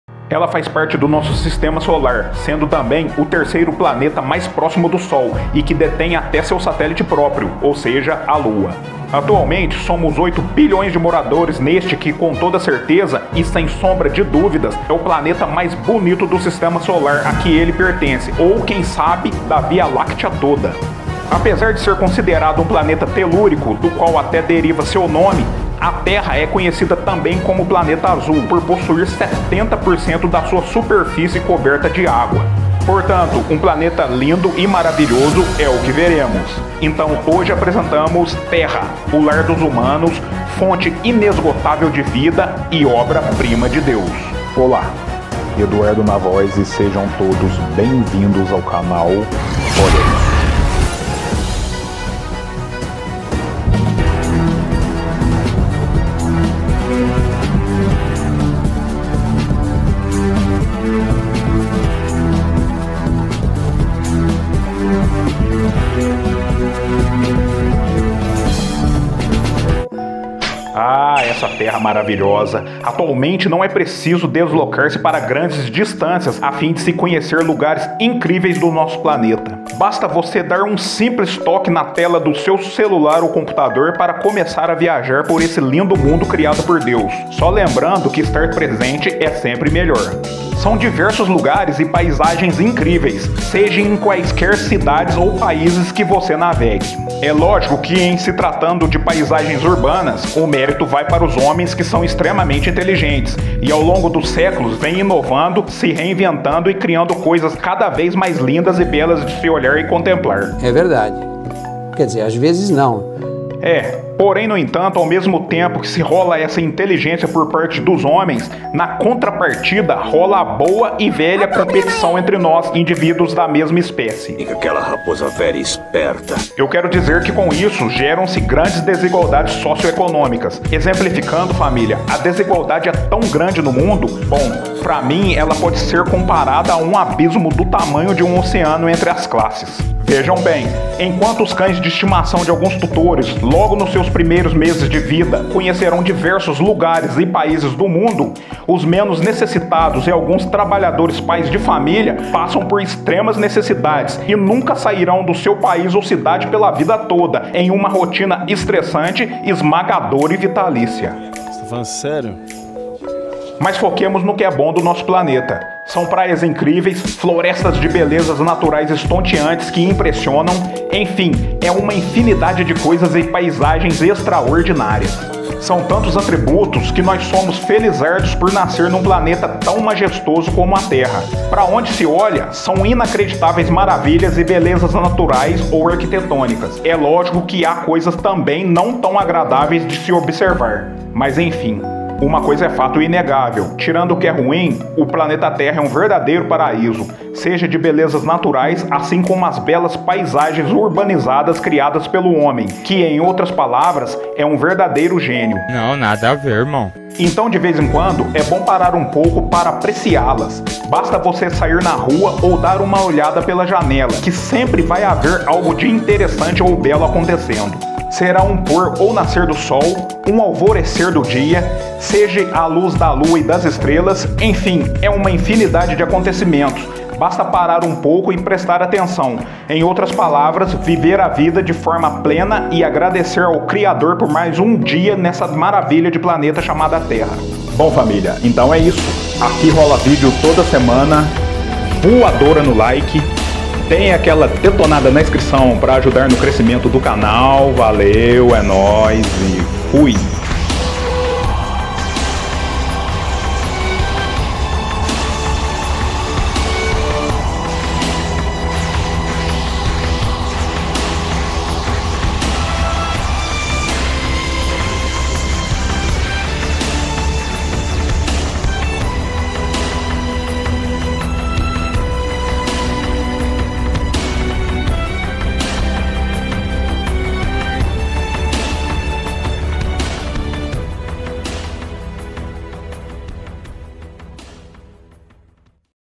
TERRA, O PLANETA MAIS BELO DO UNIVERSO!
VÍDEO SOBRE AS DIVERSAS BELEZAS NATURAIS EXISTENTES, BEM COMO AS TAMBÉM CRIADAS PELOS HOMENS.